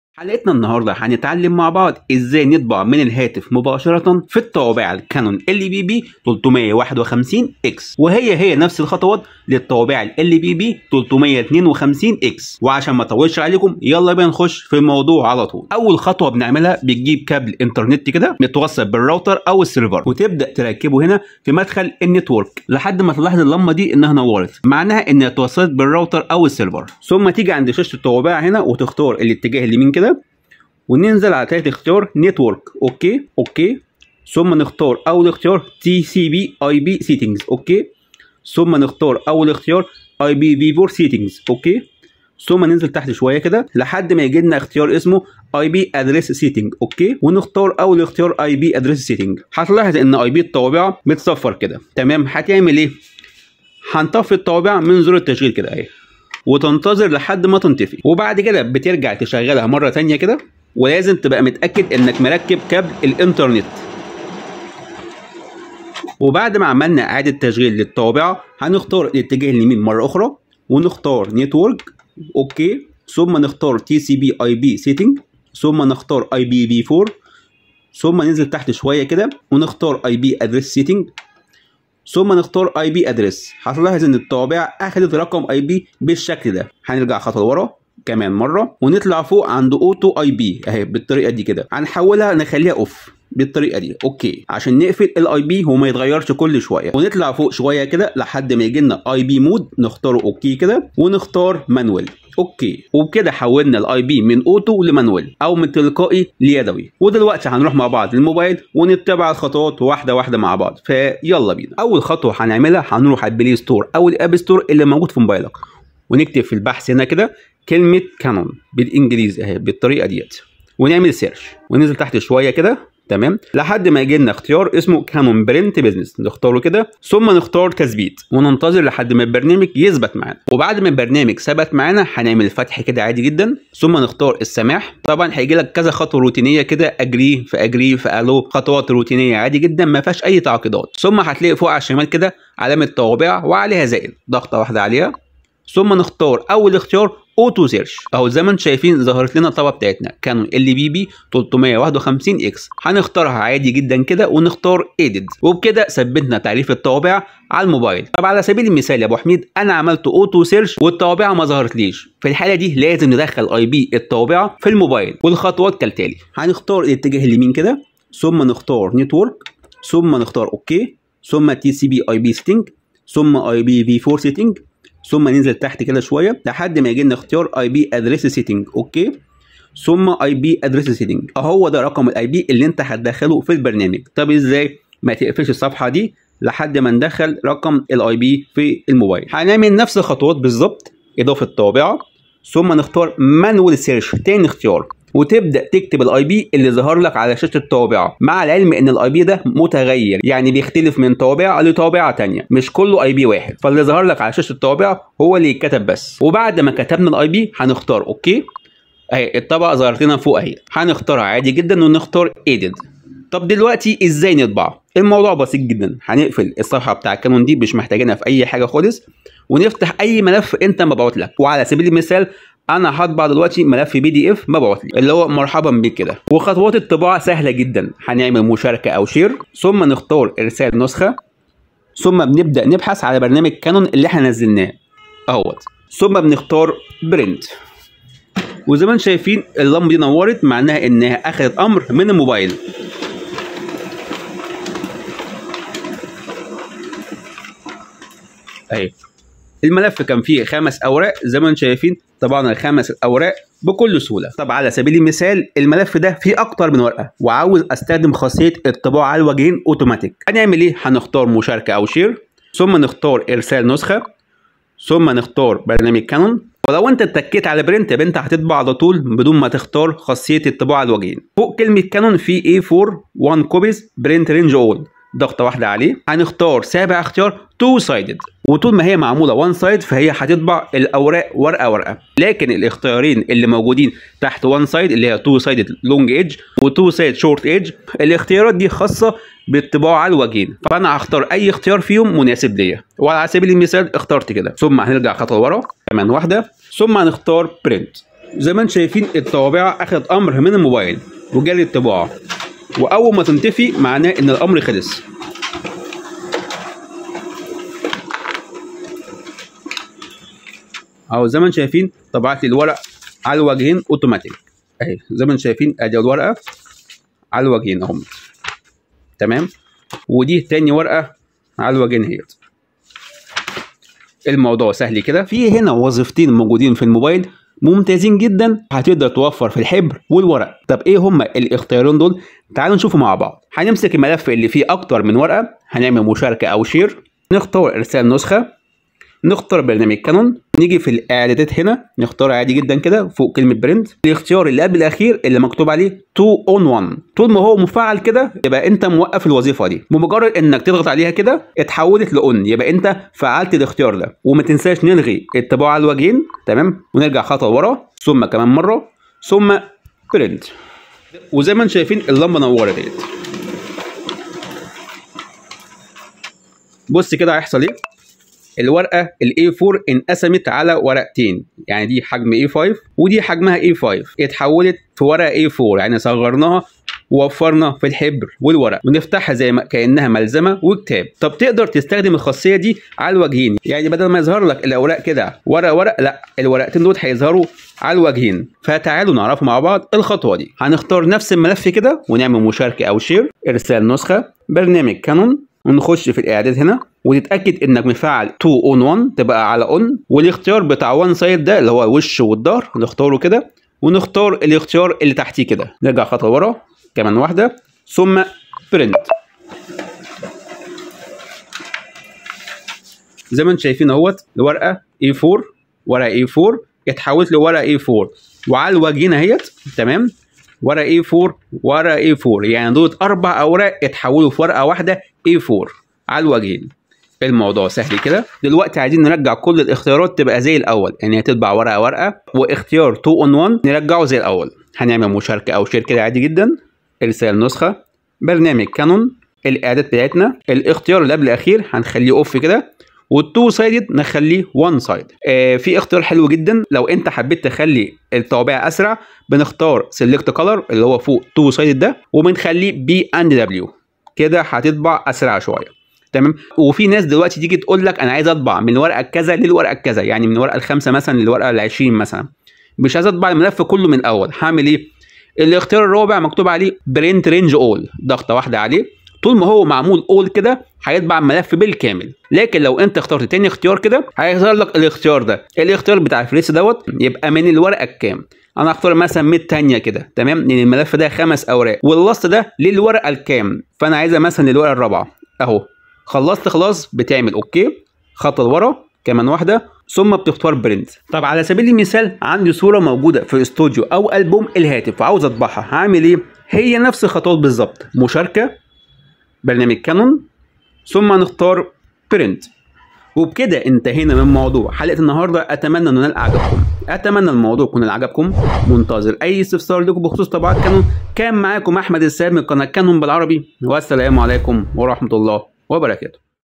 خطوات الطباعة من الهاتف على الطابعة كانون LBP351X / LBP352X
خطوات الطباعة من الهاتف على الطابعة Canon LBP351X و LBP352X

لاتنسى اللايـــــك (👍) و الأشــــتراك (👤) وتفعـــيل زر التنـبيهـات (🔔)